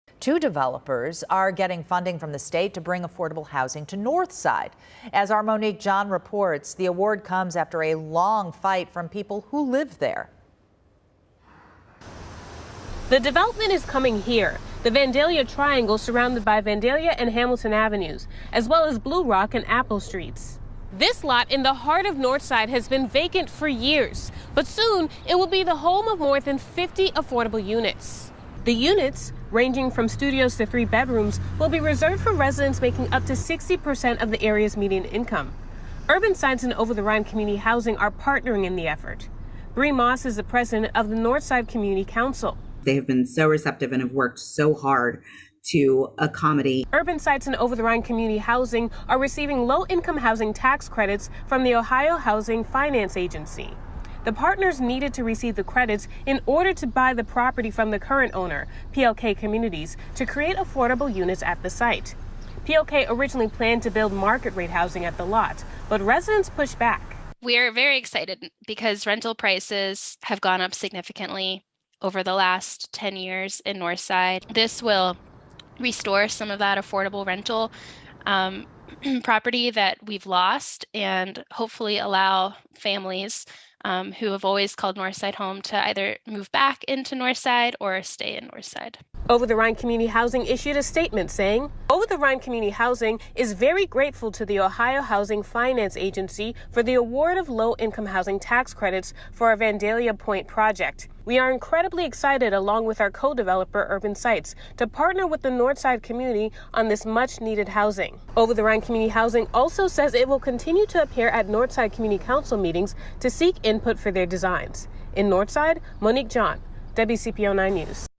Northside development gets state funds for affordable housing
Two developers received funding from the state to bring affordable housing to Northside after a long fight from residents demanding that affordable housing be built at the site of the upcoming development.